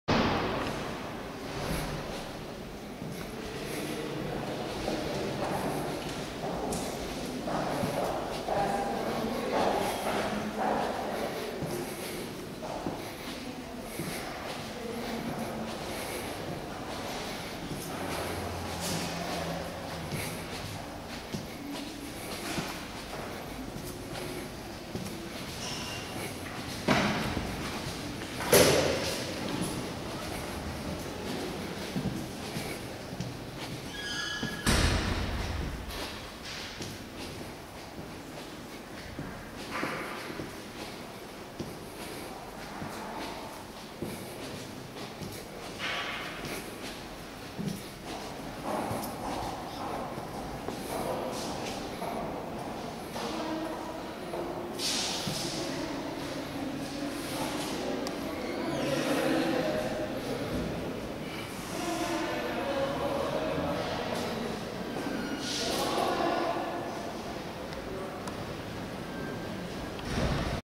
recherche de Paul Celan
Dans les couloirs de l'Ecole nationale supérieure, rue d'Ulm, qui accueillit Paul Celan.